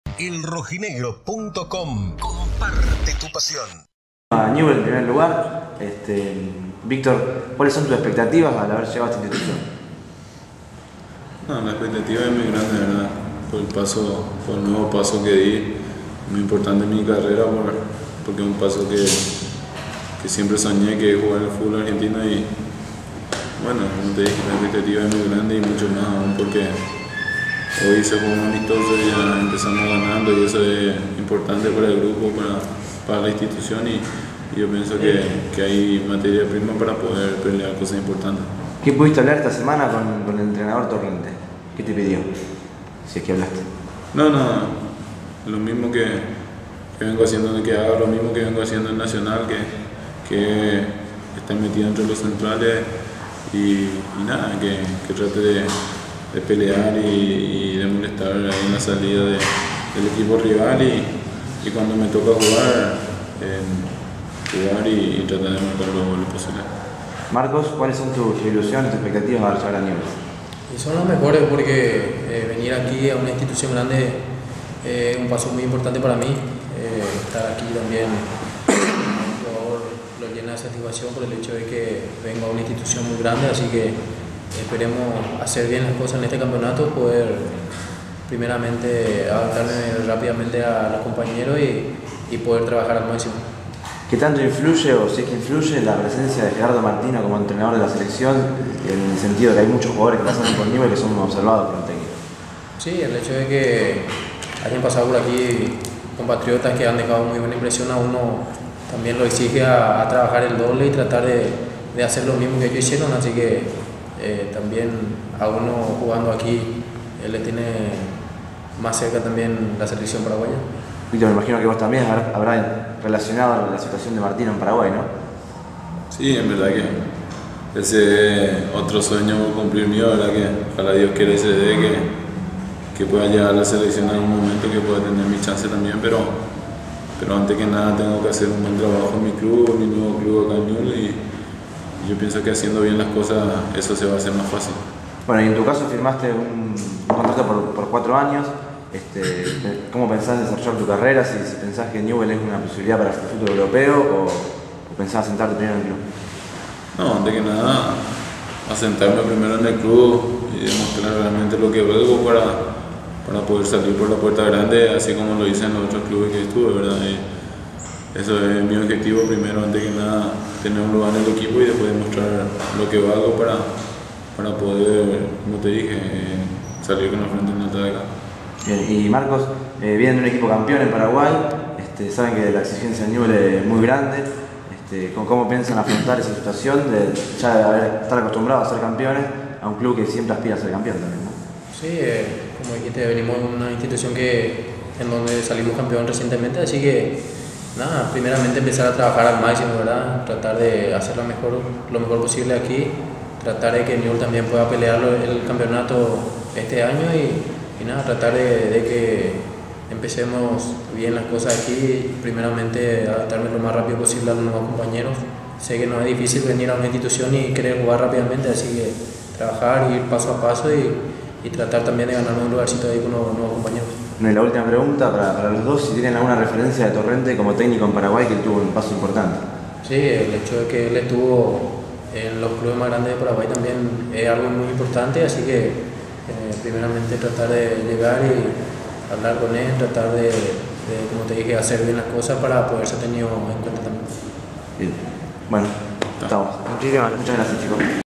ElRojinegro.com | Entrevista a Marcos Riveros y Víctor Aquino.
Los nuevos refuerzos leprosos hablaron con ElRojinegro.com .